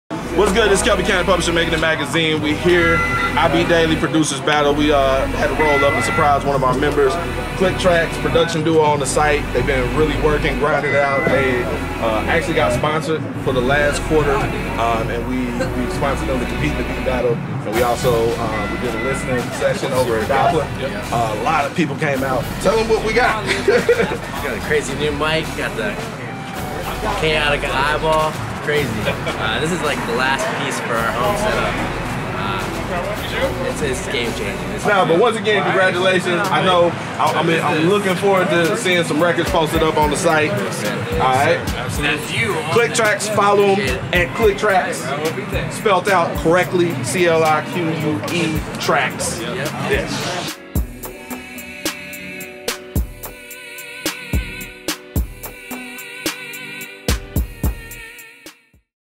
What Would You Do with an Extra $1,000 for Your Music?
Makin' It Magazine members Clique Tracks were selected for out latest Cut the Check opportunity and we paid for an equipment upgrade to complete their production studio set up including Neumann TLM 102 Studio Mic ($699 Value) and Kaotica Eyeball ($198 Value). Just another benefit of being a member

Visual produced by @Lovizionz_Media
Accompany music produced by Clique Tracks